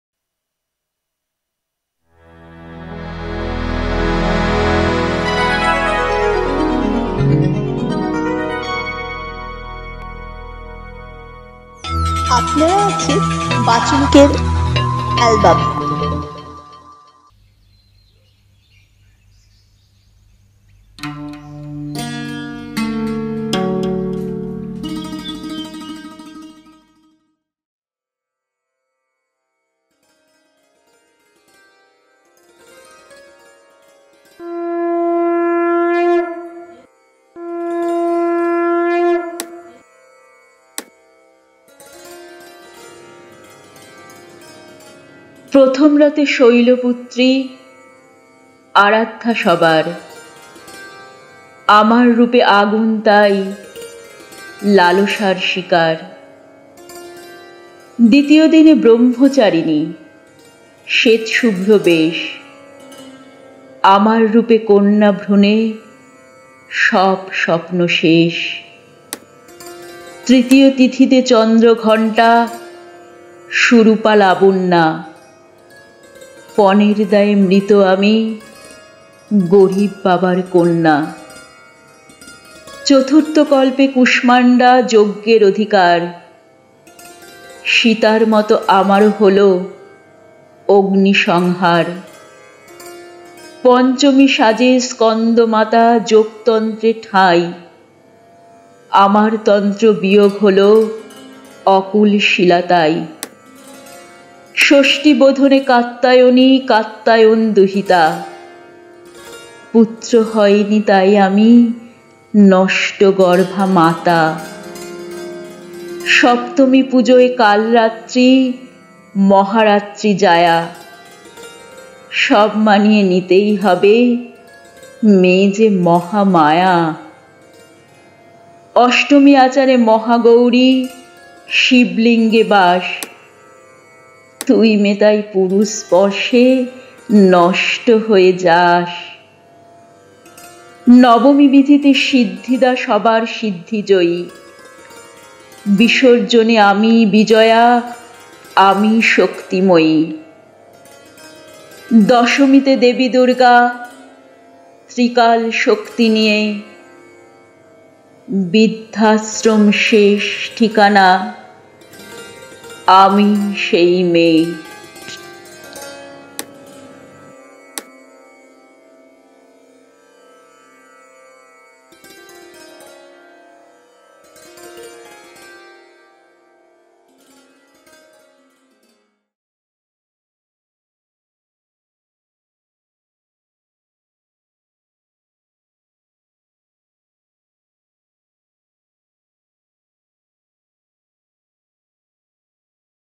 P23 Festival Yours and Mine || উৎসব তোমার আমার || Kumkum Mitra
পূজো  বিশেষ অনুষ্ঠান।
কবিতাঃ সর্বার্থসাধিকা
কবিঃ  জয় চক্রবর্তী